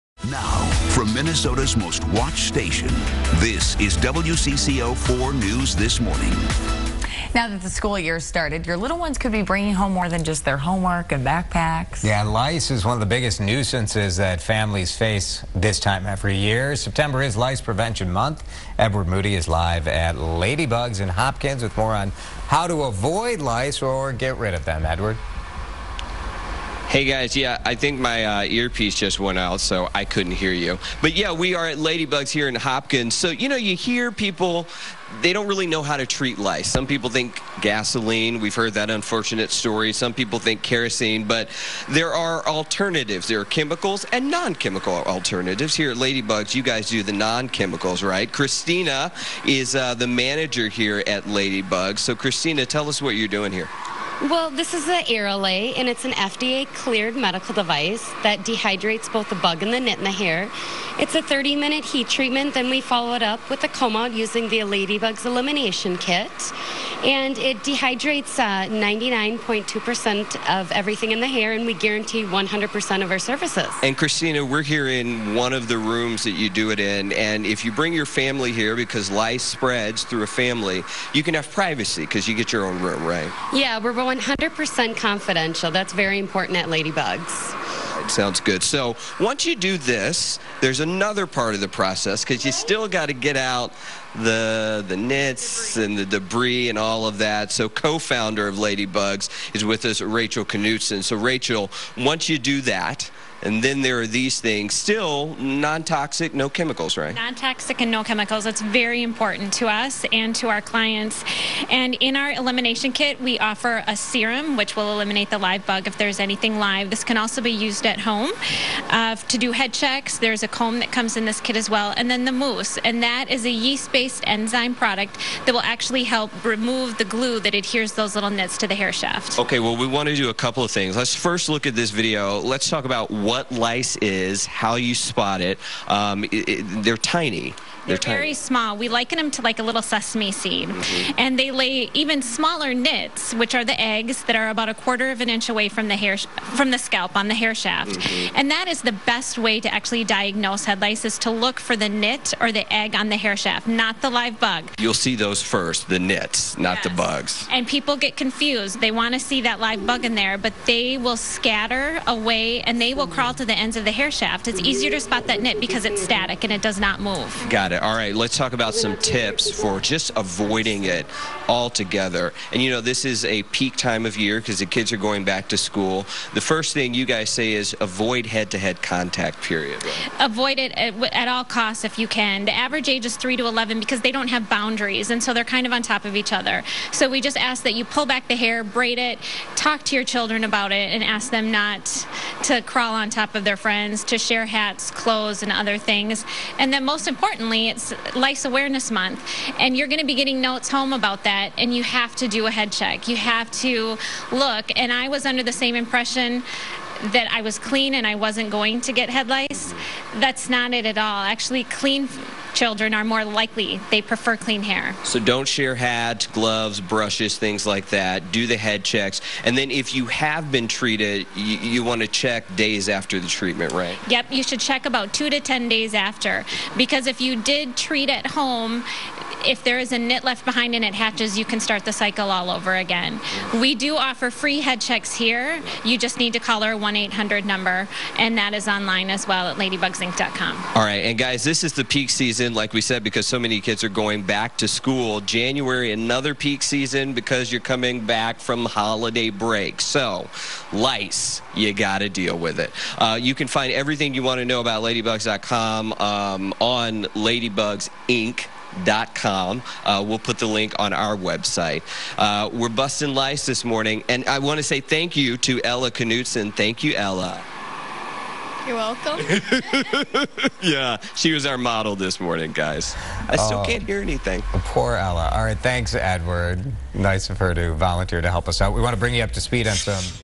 Ladibugs WCCO-TV Head Lice Awareness Month
Edward Moody from WCCO stopped at Ladibugs's Hopkins head lice center to get a few tips on how to prevent and eliminate head lice the natural way.

Remember September is head lice awareness month!